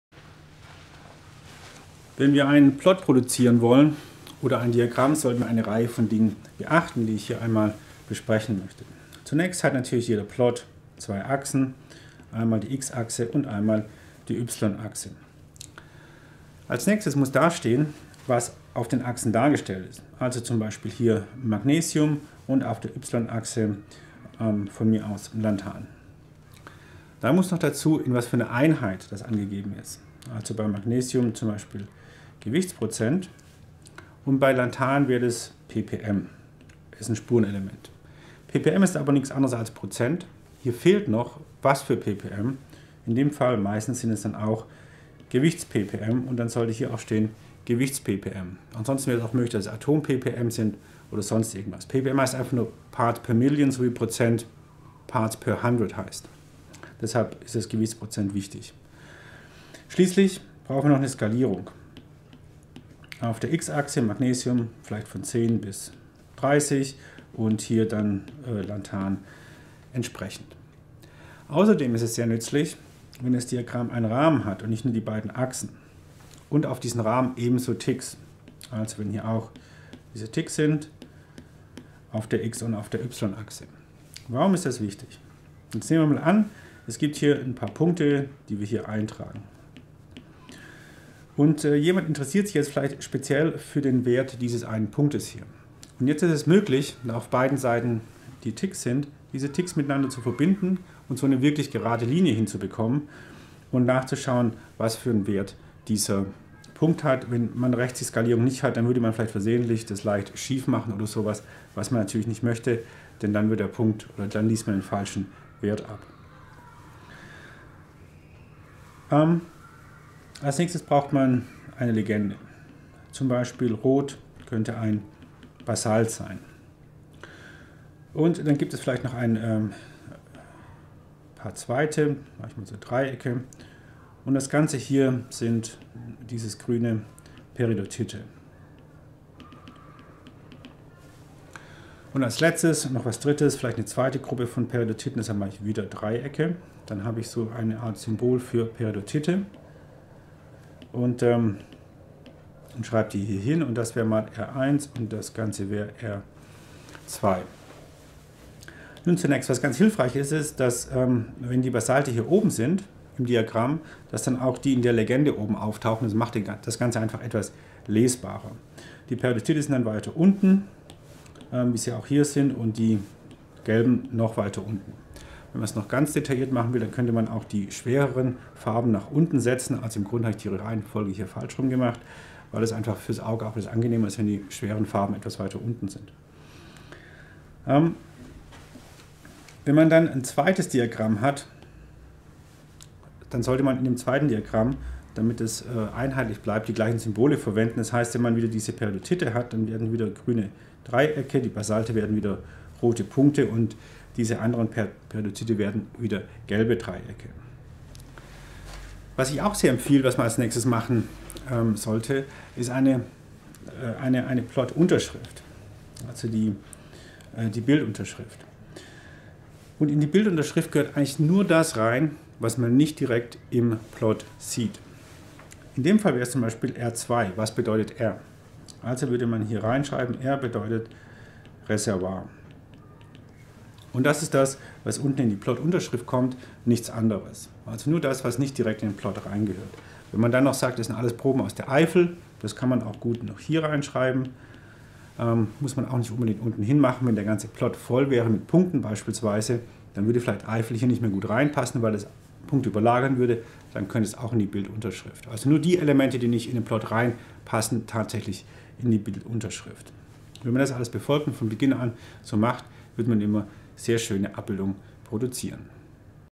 Einen Plot anfertigen
Ein kurze Anleitunge welche wichtigen Elemente eine Plot enthalten soll, bzw. darin auftauchen sollten oder müssen.